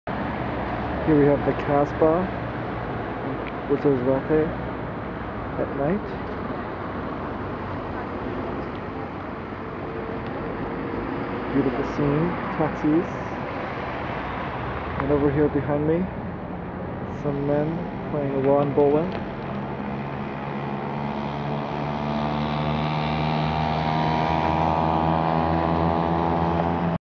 Ouarzazate Kasbah at night
A view Ouarzazate Kasbah at night, with men playing a bocce ball.